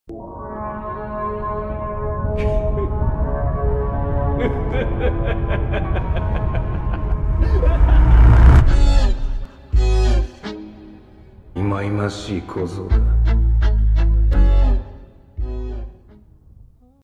Sukuna's New Vessel | Chapter 212 | Manga Animation
Made this in a few hours since the leaks were just too insane. Goku v Moro will be finished and uploaded in the coming days but until then, here's this :)